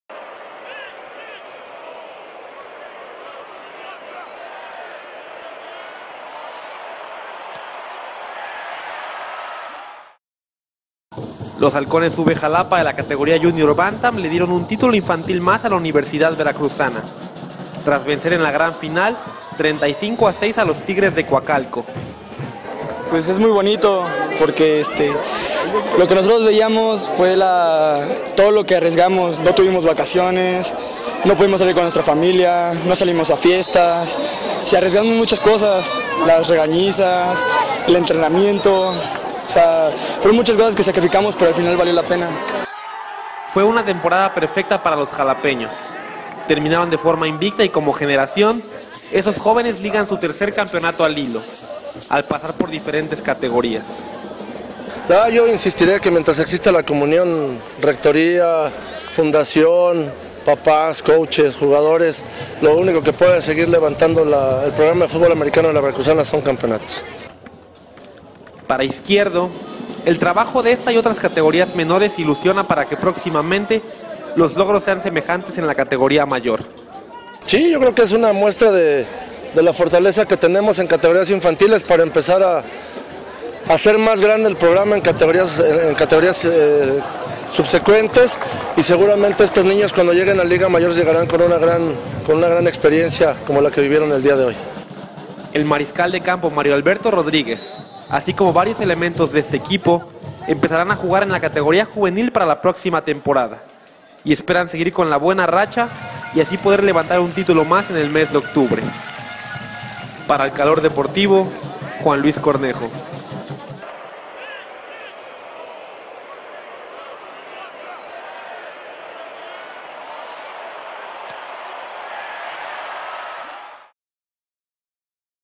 Halcones UV, campeones de la Junior Bantam
Xalapa, Ver., 5 de agosto de 2013.- Tras una temporada invicta, los Halcones UV se convirtieron en campeones de la Junior Bantam al derrotar en la gran final a los Tigres de Coacalco en el campo mixto de la USBI.